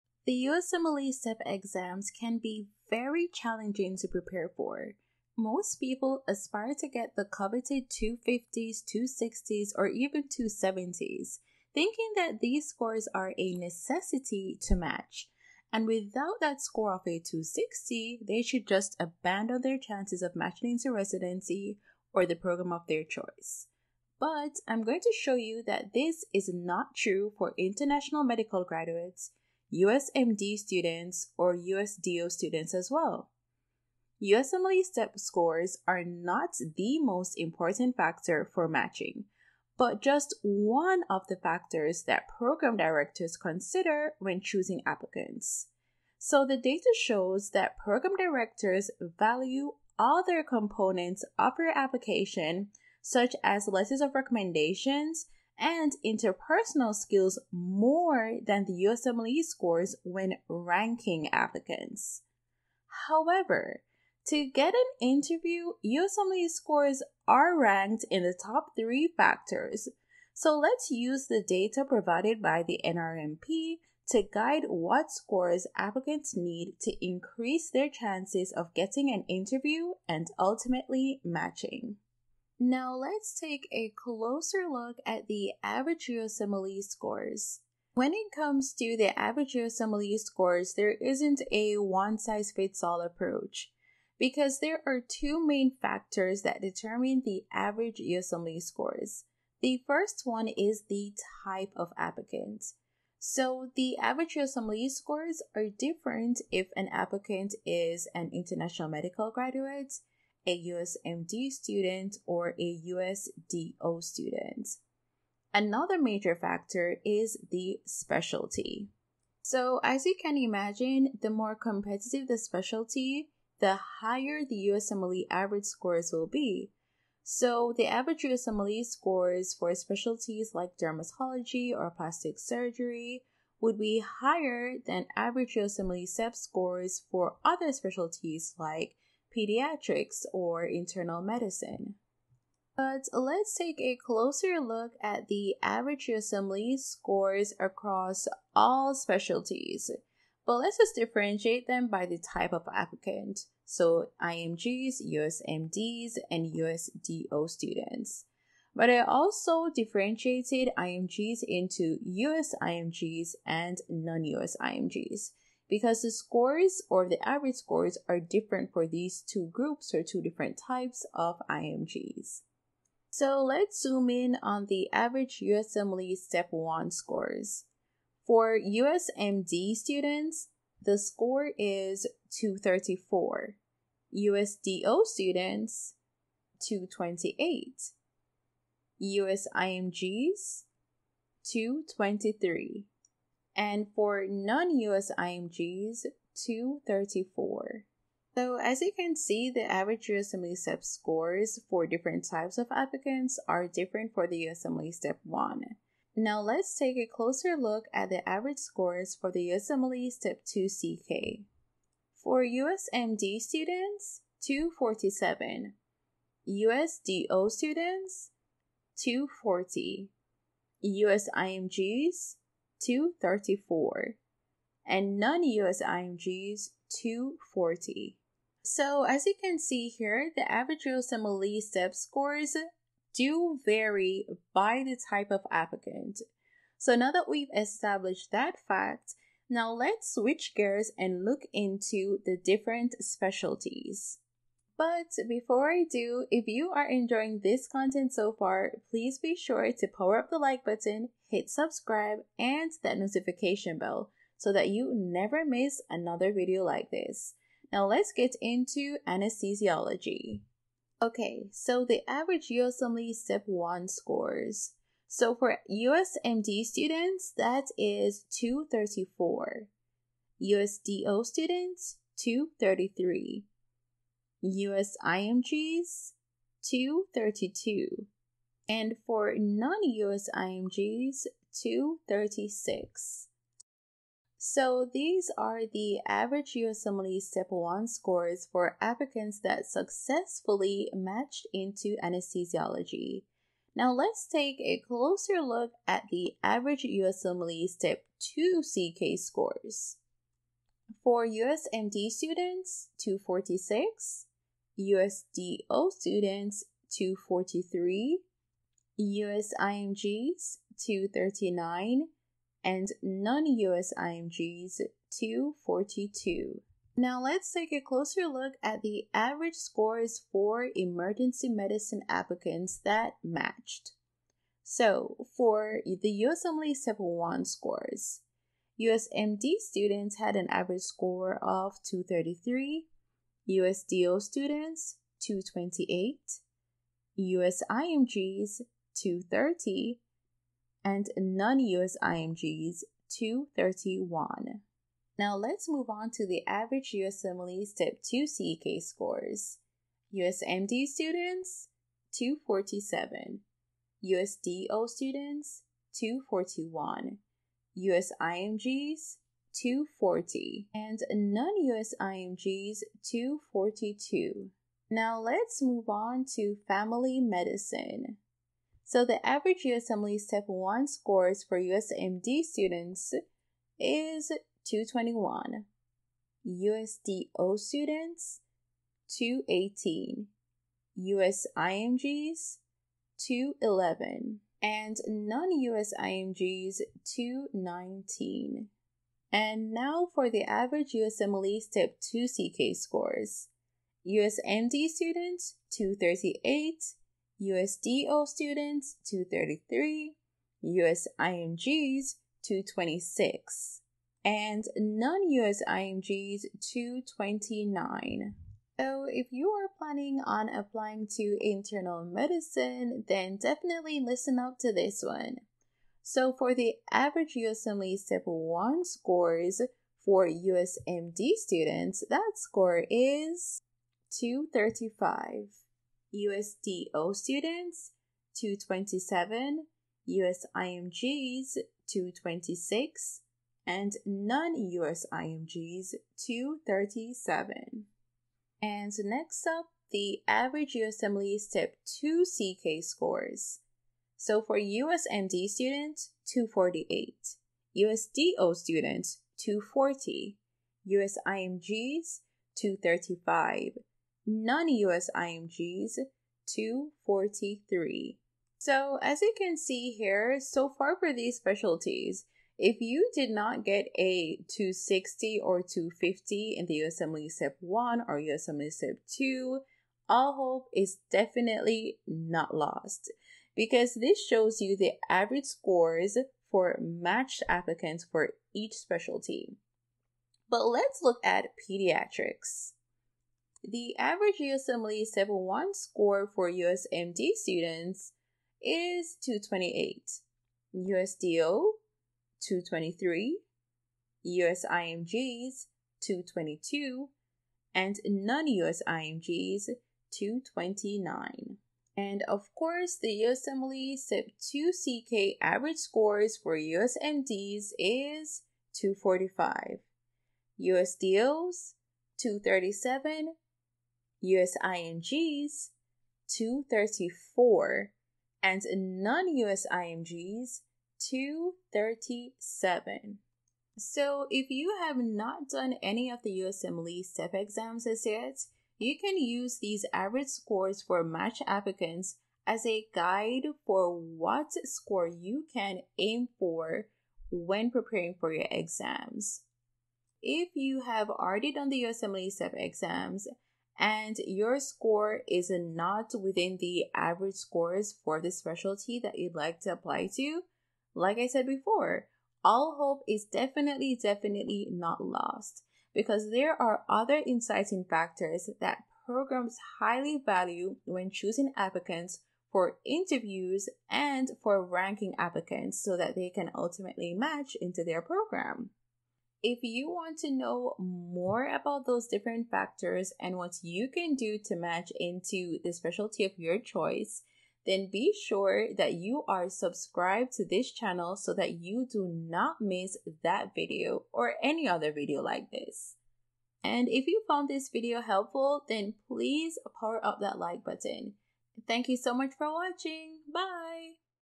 USMLE Secrets: Scores you need to MATCH!
USMLE Secrets: the real scores you need to match. USMLE Step 1 and USMLE Step 2 CK Scores you need to increase your chances of getting an interview, ranking and ultimately matching varies by the type of applicant and the specialty.
In this video, I will answer questions like:
What USMLE Step 2 CK score do I need to match?
What is a 'good' USMLE score for IMGs?
What scores do I need to match in Internal medicine?

0:00 Why you don't need a 250 to match
1:23 Overall average USMLE scores for IMGs, US MD, US DO
4:10 Average USMLE Scores by Specialty